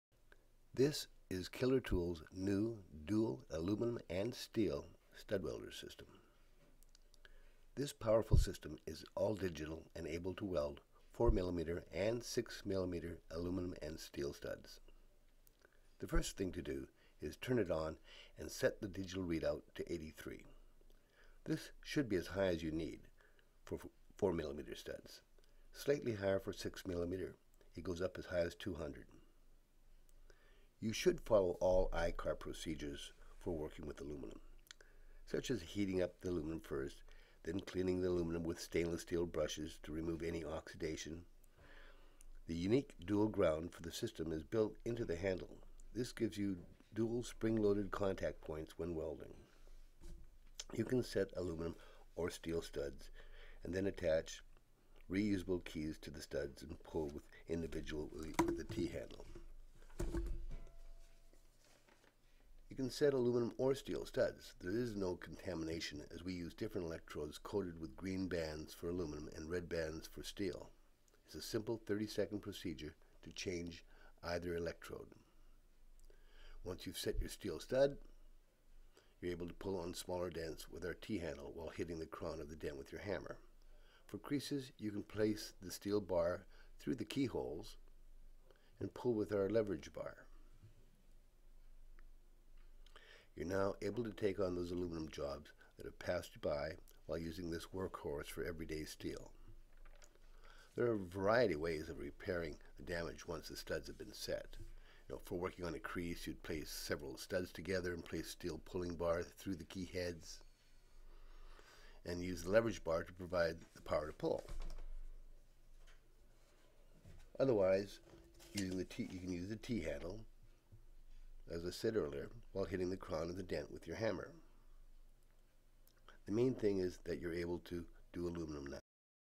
ART401 Aluminum and Steel Stud Welder
A demonstration on how to use Killer Tools' Aluminum Stud Welder.